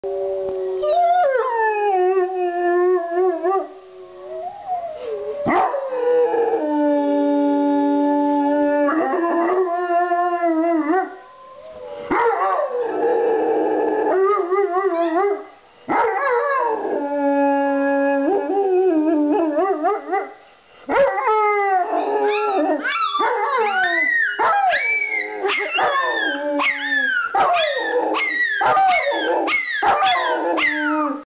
Rottweiler and Pomeranian Funny Howling
My dogs, rottweiler and pomeranian howling to the sound of wolves howling on the computer. My pomeranian's screechy howl is hilarious.